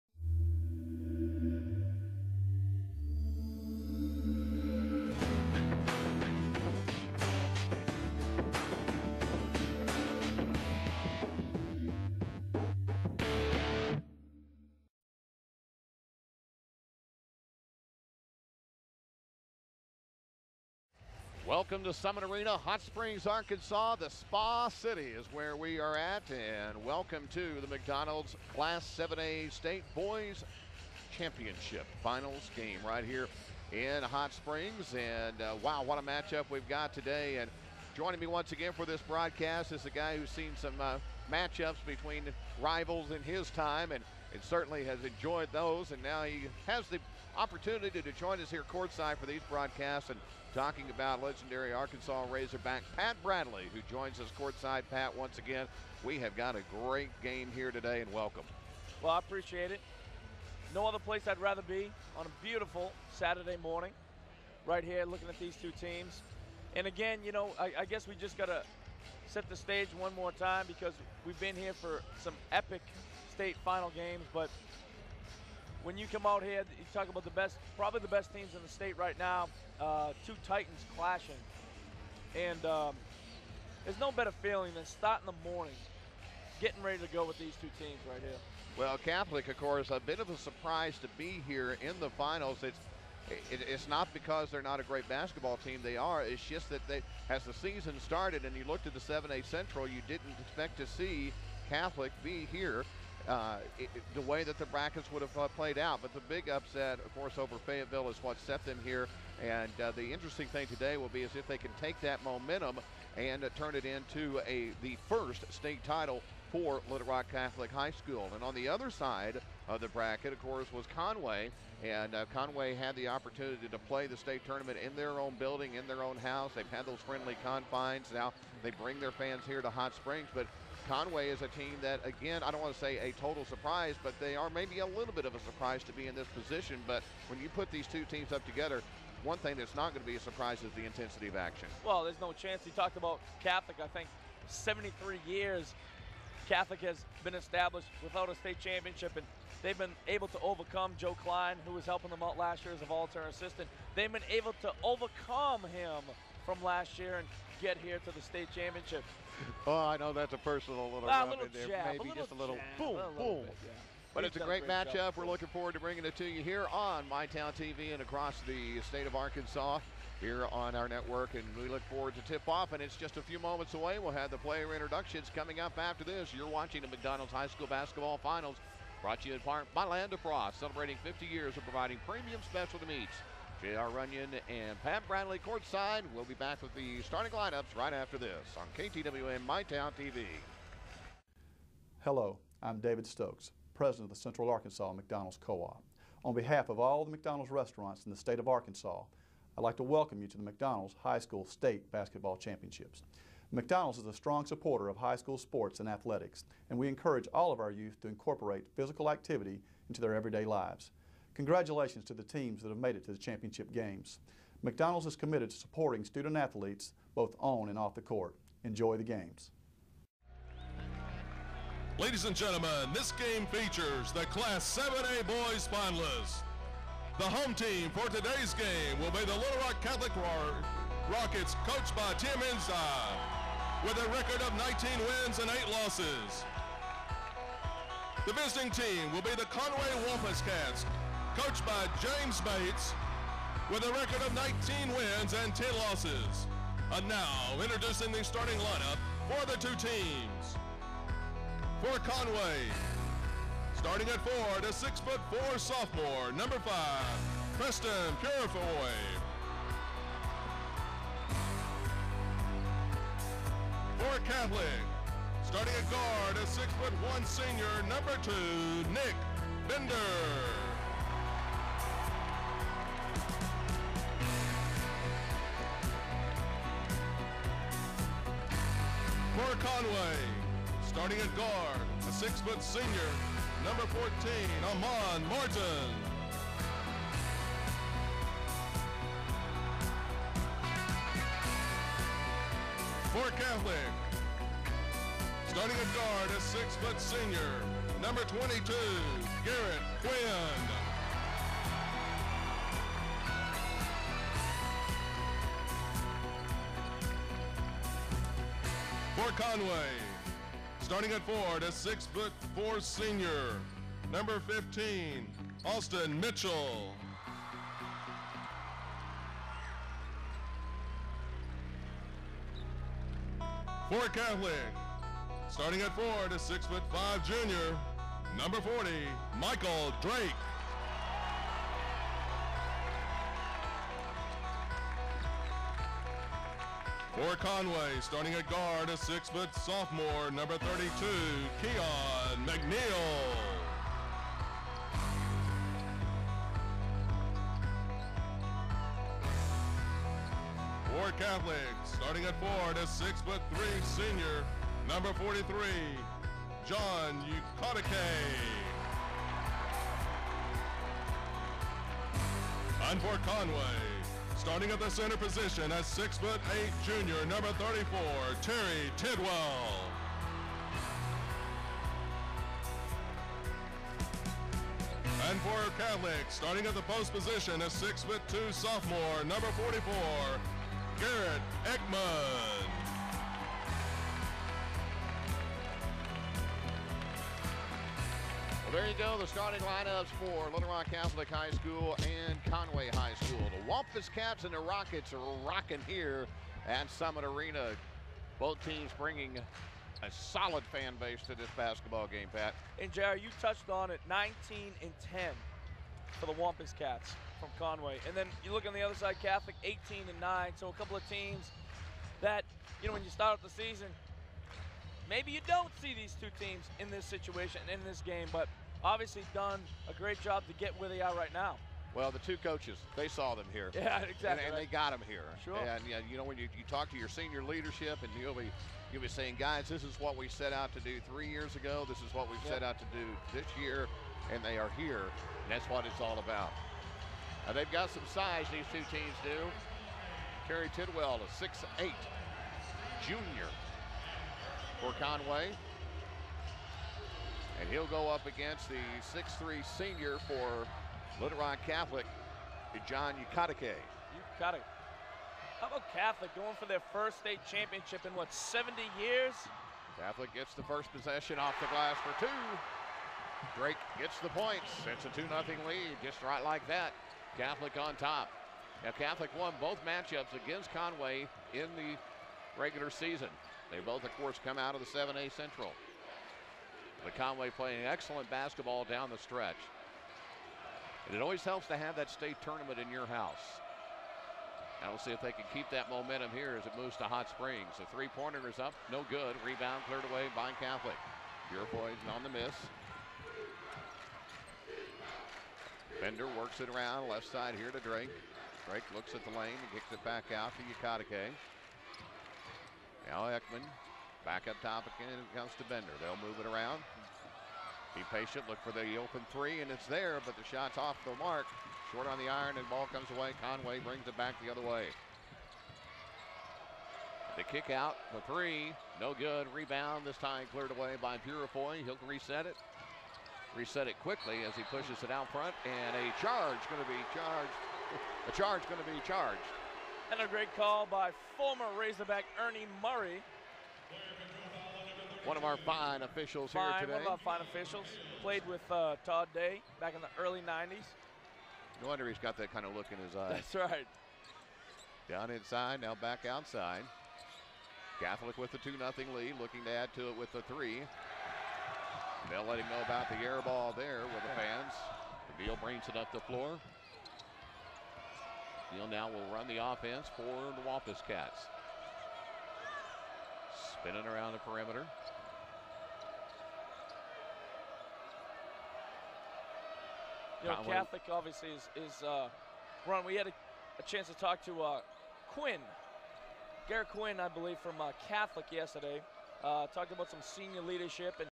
Catholic State Basketball 2008
Little Rock Catholic High School state basketball championship vs Conway High School 2008